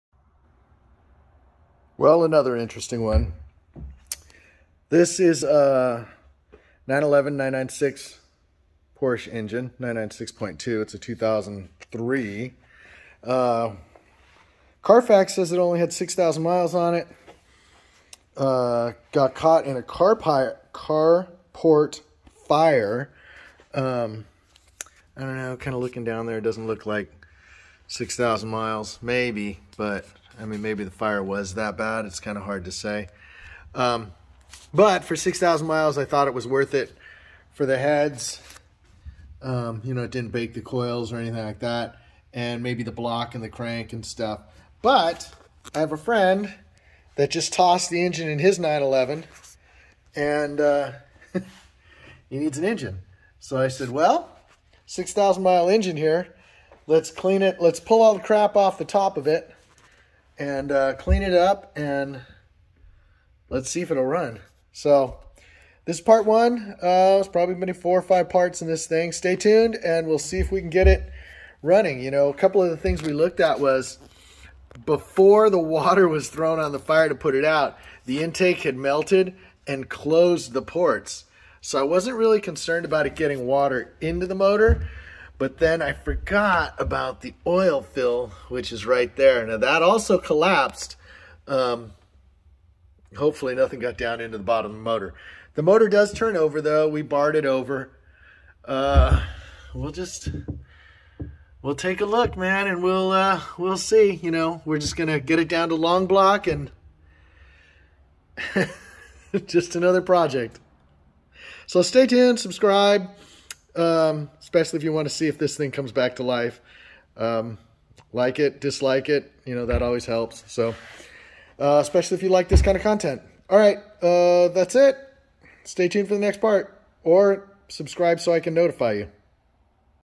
Porsche 911 996 997 Copart Burn Job Will it Run After Fire DIY Engine Swap
I bought this Porsche 911 4S 996.2 engine a for a core because the Carfax said it is an original 6k mile engine.

Coincidentally, my friend just blew up the engine in his 4S so I offered him this one if it's a reasonable thing to do.  That's all going to depend on how it looks once we get all the melted plastic off of it and take a look under the covers.